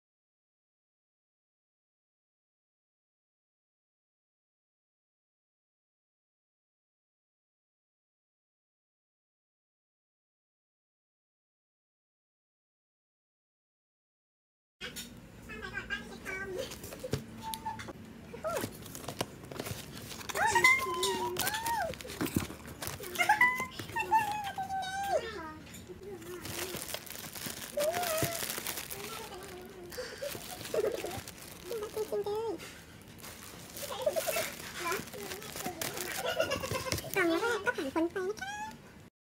Won Cony the Unicorn (Luck)
For more pics and video

Won midium Cony the Unicorn from Toreba

I didn’t think I will get it I think tho
Think it’s just my luck 🤣😆 yayy